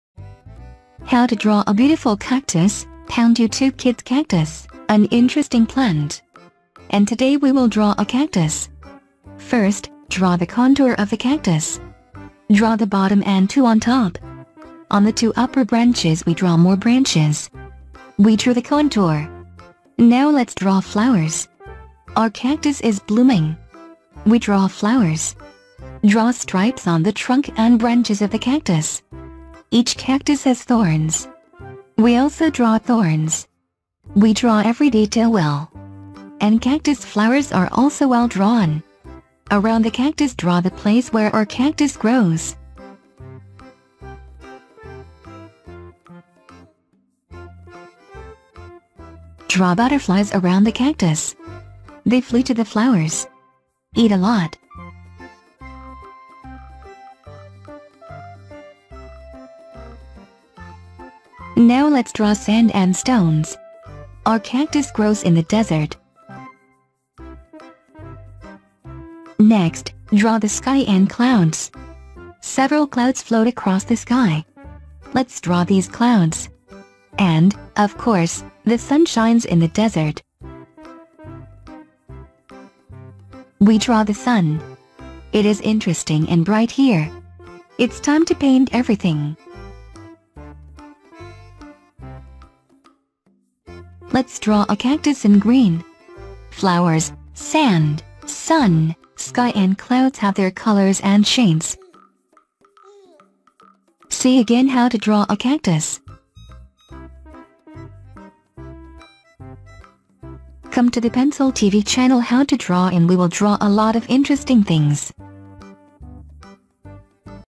How to draw a beautiful cactus
Cactus - an interesting plant. And today we will draw a cactus.
First, draw the contour of the cactus. Draw the bottom and two on top.
On the two upper branches, we draw more branches. We drew the contour.
Now let's draw flowers. Our cactus is blooming. We draw flowers.
Draw stripes on the trunk and branches of the cactus.
Each cactus has thorns. We also draw thorns.
We draw every detail well. And cactus flowers are also well drawn.
Around the cactus draw the place where our cactus grows.
Draw butterflies around the cactus. They flew to the flowers. Eat a lot.
Now let's draw sand and stones. Our cactus grows in the desert.
Next, draw the sky and clouds. Several clouds float across the sky. Let's draw these clouds.
And, of course, the sun shines in the desert. We draw the sun. It is interesting and bright here.
It's time to paint everything. Let's draw a cactus in green.
Flowers, sand, sun, sky and clouds have their colors and shades.
See again how to draw a cactus.
Come to the Pencil TV channel How to draw, and we will draw a lot of interesting things.